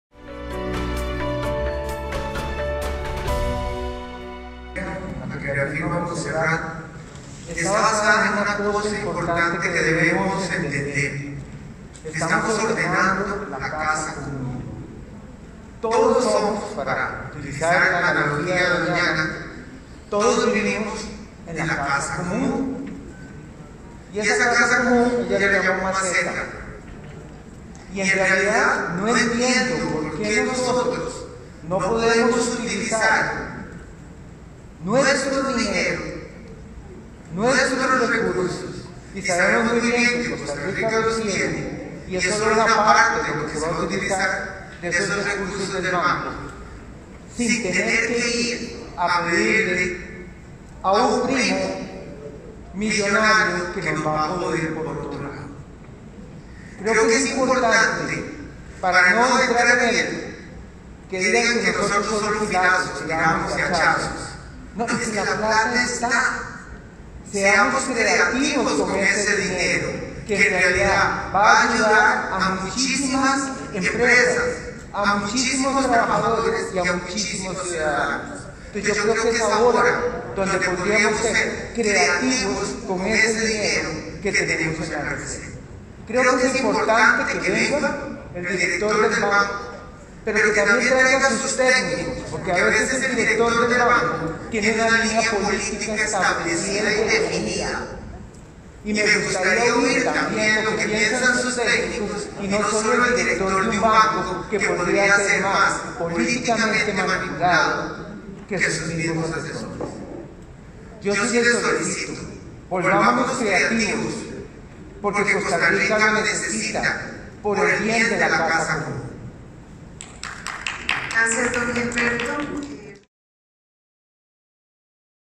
Participación del Presidente de ANDE Mesa de diálogo multisectorial
Participación del Presidente de ANDE
Mesa de diálogo multisectorial

“Costa Rica necesita que se ordene la casa”